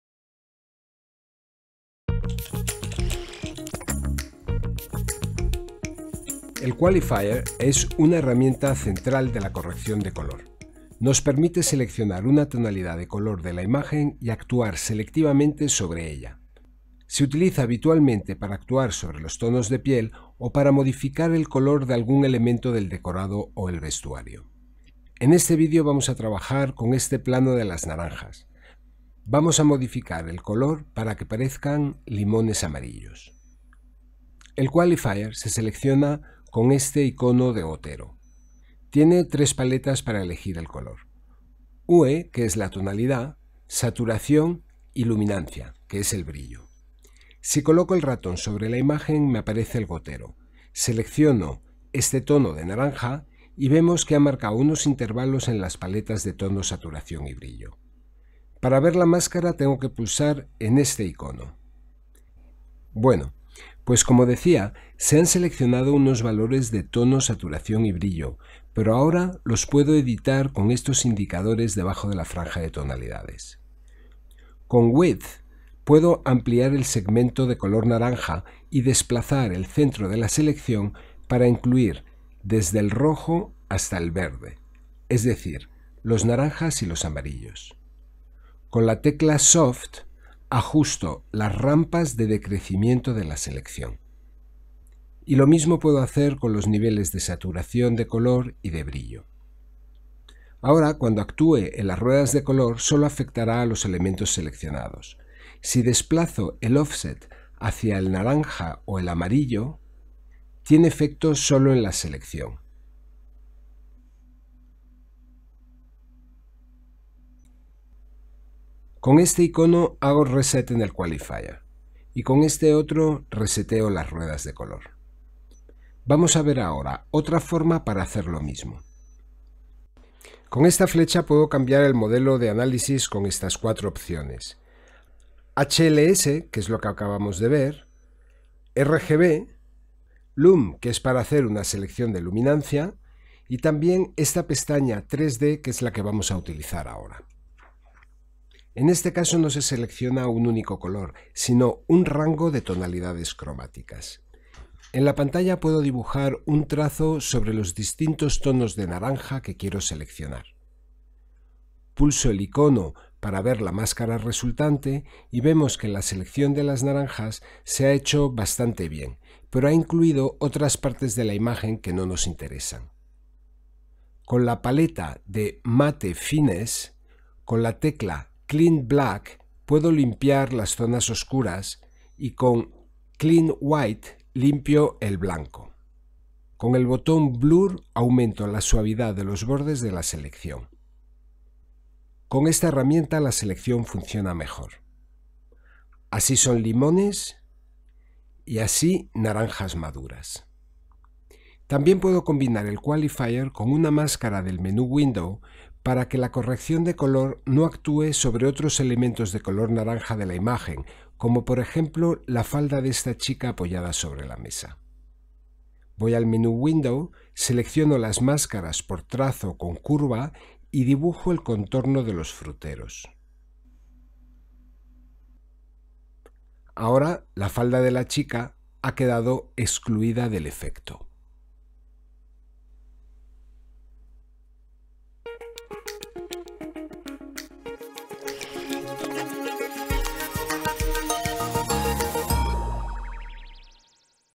6. El qualifier en la la pantalla de color de DaVinci
En este vídeo vamos a ver la herramienta Qualifier que sirve para hacer mascaras a partir de la selección un color. Es el mismo procedimiento que se usa para hacer la incrustación de crominancia,  pero en esta ocasión orientado a la corrección de color.

Para seguir este ejercicio puedes descargar los materiales de este enlace